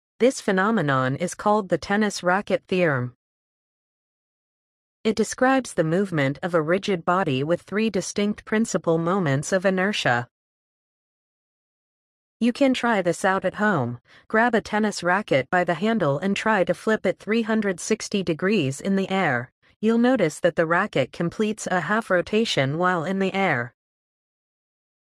The Tennis Racket Theorm, or the Dzhanibekov effect
The Tennis Racket Theorm, or the Dzhanibekov effect, is one of many physics quirks that can be demonstrated on the international space station! 🚀🤙

For any Copyright Related matters (credits or video removal)